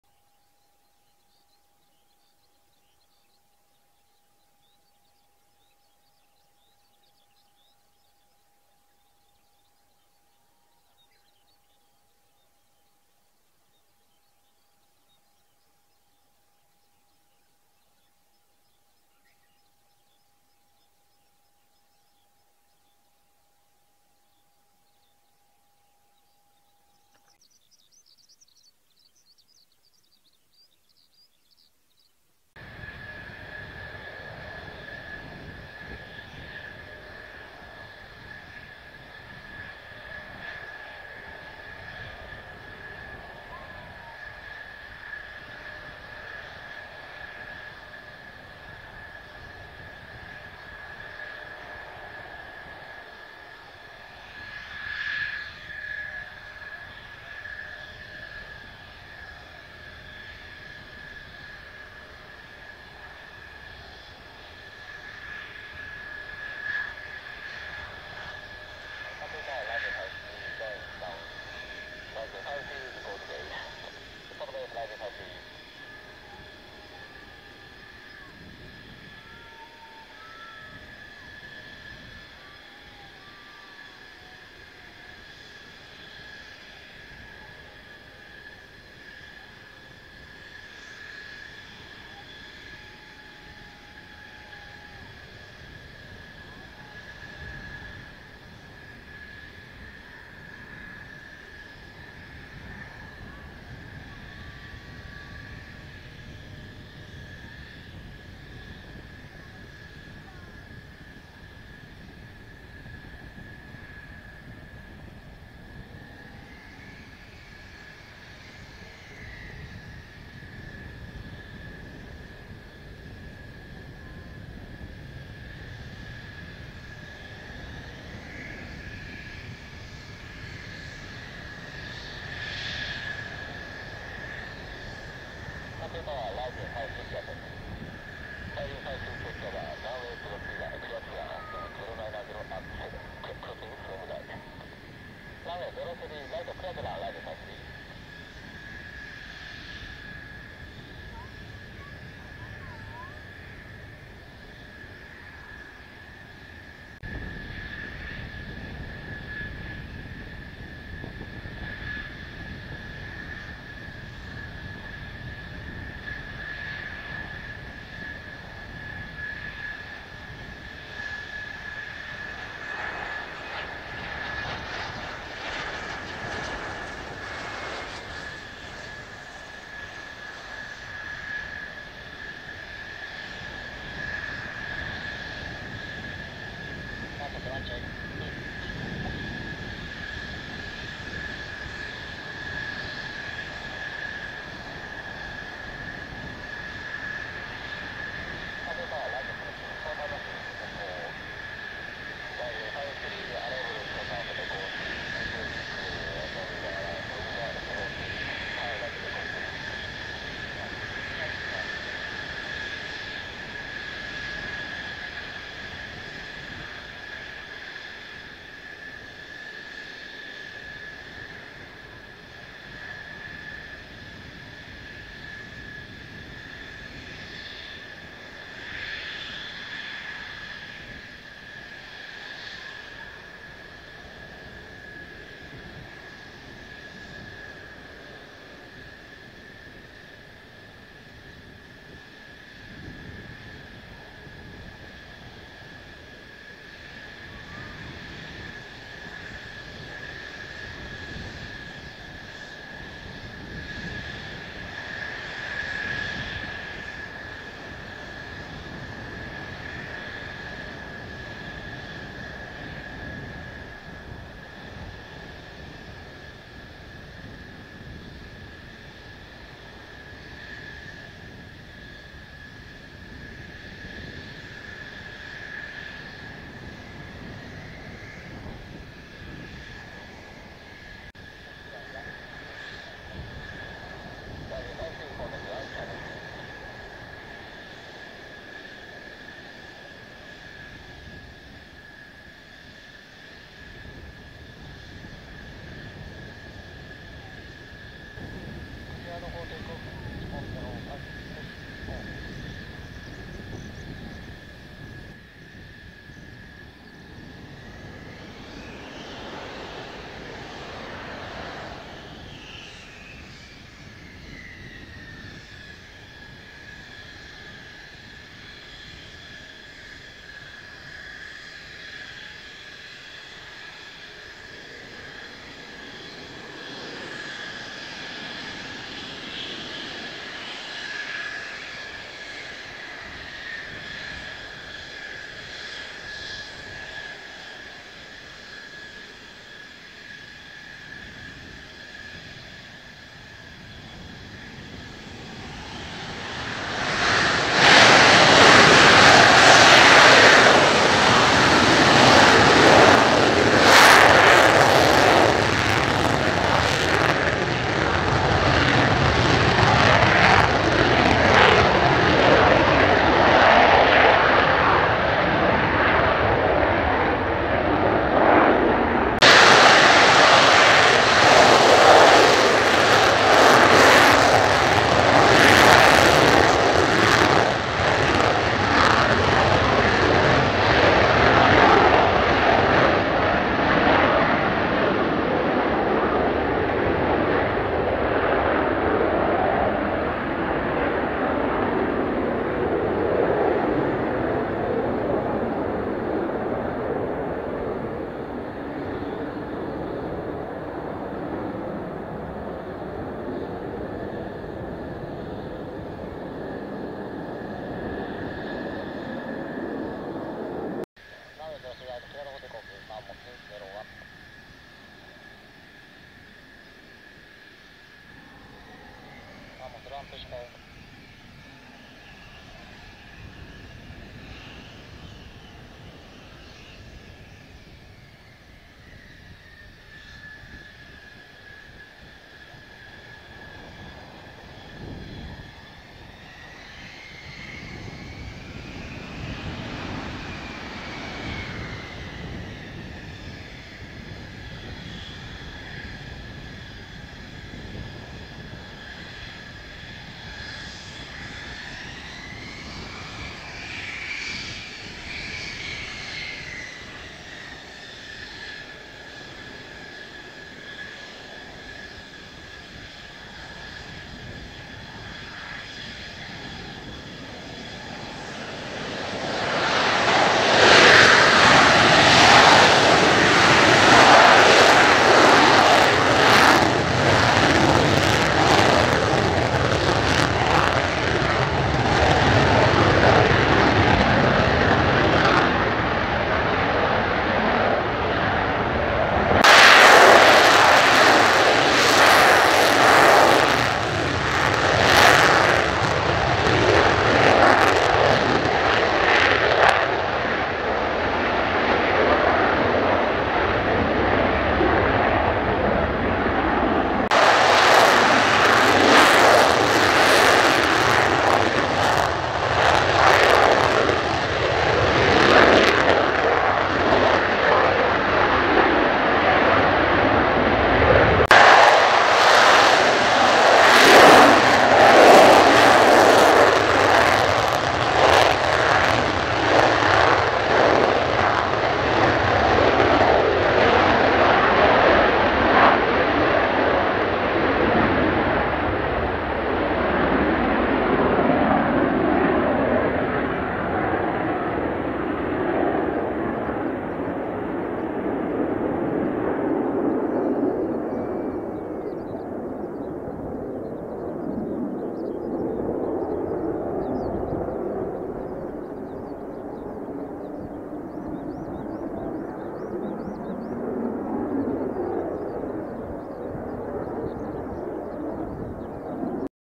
陽炎メラメラ!!百里基地F-2戦闘機!!洋上迷彩までユラユラ波打つほど…。1stファースト飛行訓練はお昼前からでゆっくりめ♪でも6機飛んだ!!サンスコ/3sq/JASDF
航空自衛隊百里基地[第7航空団]

*第3飛行隊(3rd sq)
Mitsubishi F-2A(Fighter)

☆撮影日/Shooting Date
6月1日/Jun,01,2021

※超望遠1000mm手持ち撮影ですので多少の手振れ、フレームアウト、風切り音(ウインドスクリーン装着済み)、フォーカスアウト、地面傾きなどが発生しております。ご理解とご了承をお願いします。